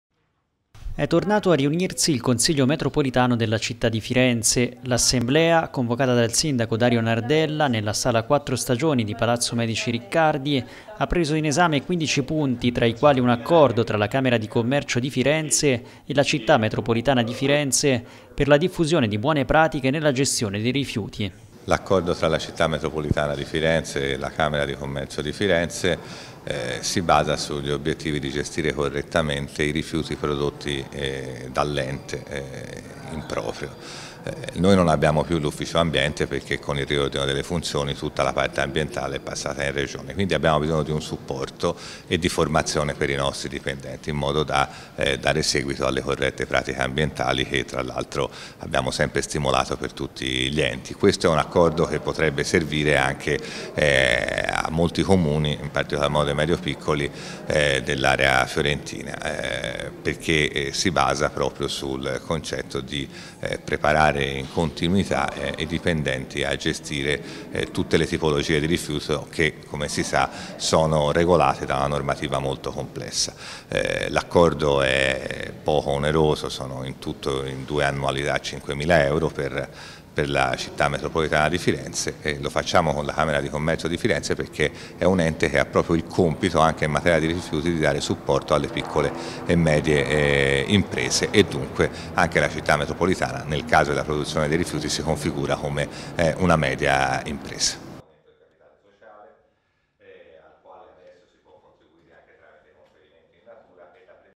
Firenze, nuovo accordo per le Buone pratiche nella gestione dei rifiuti
Firenze, siglato un accordo tra Camera di Commercio e Città Metropolitana per la diffusione di buone pratiche nella gestione dei rifiuti

Servizio di Michele Francalanci; Intervista a Massimiliano Pescini - sindaco di San Casciano Val di Pesa